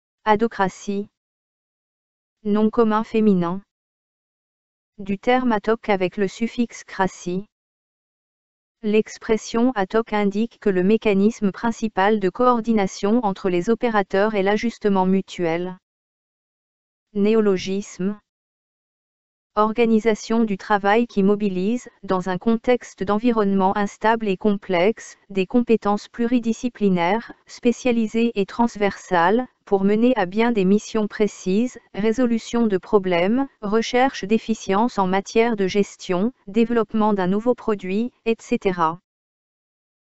Adhocratie | Dictionnaire français audio
Définition du terme Adhocratie.
Du terme ad hoc avec le suffixe -cratie. L’expression ad hoc indique que le mécanisme principal de coordination entre les opérateurs est l’ajustement mutuel.
Néologisme. Organisation du travail qui mobilise, dans un contexte d’environnements instables et complexes, des compétences pluridisciplinaires, spécialisées et transversales, pour mener à bien des missions précises (résolution de problèmes, recherche d’efficience en matière de gestion, développement d’un nouveau produit, etc.).